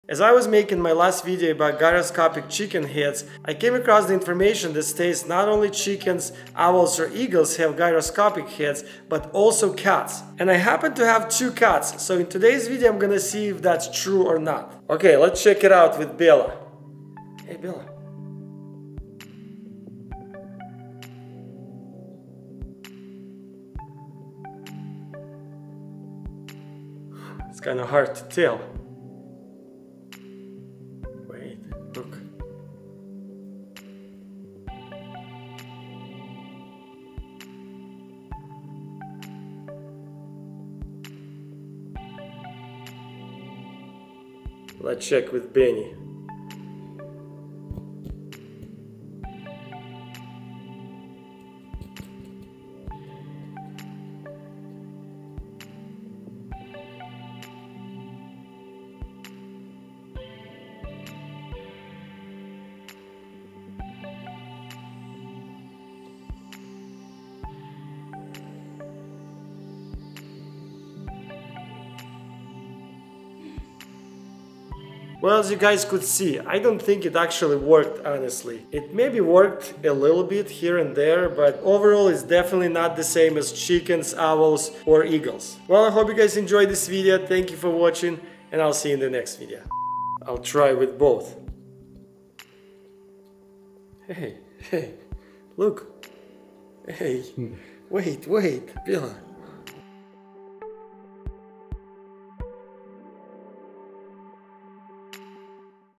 Do CATS Have GYROSCOPIC Heads? Let's Find Out!
We all know that owl, eagles, chickens have gyroscopic heads! Some state that cats as well do have that ability! In today's video I'm testing this statement about cats having gyroscopic heads! Let's found out! Science experiment!

Mad Russian Scientist